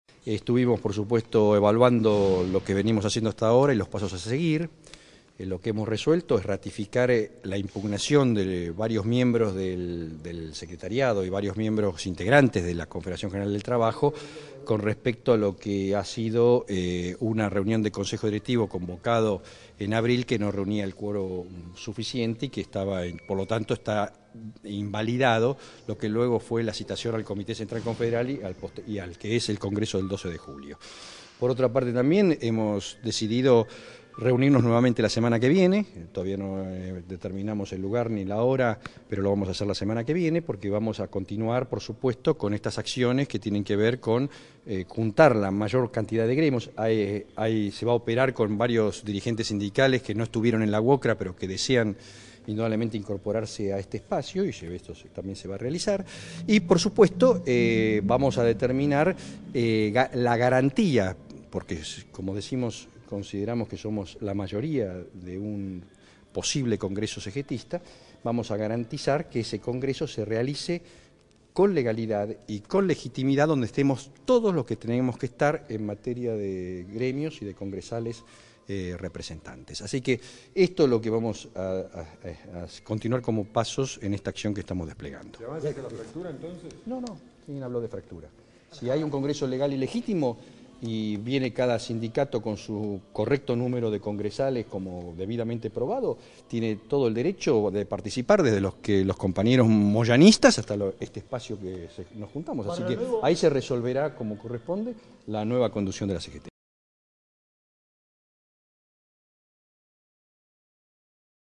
Opositores a Moyano impugnarán convocatoria al Comité Central Confederal de la CGT
El titular de la Unión del Personal Civil de la Nación (UPCN), Andrés Rodríguez, aseguró esta noche que "se ratificó" la impugnación de la convocatoria al Comité Central Confederal de la CGT y confirmó que los gremialistas opositores a Hugo Moyano realizarán una próxima reunión para reunir "la mayor cantidad de sindicatos que deseen incorporarse".

"Se reiteró la convocatoria del encuentro de la semana pasada en la sede de la UOCRA, que reunió a más de 75 gremios", precisó el dirigente a la prensa, y aseguró que "se ratificó la impugnación del Confederal, que no logró el quórum necesario".

Agregó que se decidió realizar otra reunión la semana próxima para continuar juntando "la mayor cantidad de gremios que deseen incorporarse a este espacio" y subrayó que se procurará garantizar que el Congreso se realice "con la legitimidad que corresponde".

"No se puede hablar de ruptura en la CGT si se realiza un Congreso legal y legítimo con su correcta cantidad de delegados", afirmó Rodríguez en la sede de la UPCN, en Moreno al 1.300 de la Capital Federal, donde además apuntó que Antonio Caló no es aún el postulante del sector, ya que "antes de hablar de un candidato hay que referirse a la legalidad del Congreso".

El titular del Sindicato de Luz Fuerza, Oscar Lescano, reclamó la confección de padrones "como corresponde" y se quejó de que el Confederal del 23 de mayo, convocado por Moyano, aceptó la afiliación de numerosos sindicatos que "este sector no conoce".

"Moyano no tiene los congresales necesarios para la reelección. Caló es el candidato de este sector y, el modelo futuro de conducción de la CGT, debe ser colegiado", puntualizó Lescano, quien tampoco descartó la posibilidad de "un triunvirato".

El dirigente gremial explicó que, en ese caso, ya expresó su deseo de integrarlo el gastronómico Luis Barrionuevo.

"Ojalá que sea posible participar en el Congreso Extraordinario del 12 de julio" próximo, afirmó Lescano, quien concluyó señalando que cuando "se observe cuántos quedan del lado de Moyano todos se darán cuenta de que no hay división ni habrá dos CGT".

Lescano aseguró el lunes último la imposibilidad de participar en el Congreso de julio porque terminará en "una batalla campal".

Además, el titular de la Unión Tranviarios Automotor (UTA), Roberto Fernández, subrayó de forma previa a la reunión que el sector procurará hasta último momento lograr "la unidad obrera" y "no dejar herido a nadie".

Entre otros, participaron además Carlos Acuña (estaciones de servicio), Jorge Viviani (taxistas), José Lingeri (obras sanitarias) y Armando Cavalieri (mercantiles), quien coincidió en la necesidad de impugnar el Confederal.